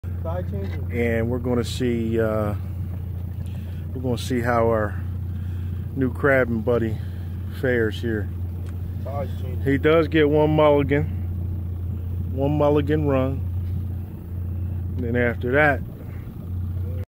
Crabbing New Jersey Style
Crabbin and Fishin is our fun time.  We love taking newbies out to show them how we do it.  Check out the full video in the Library.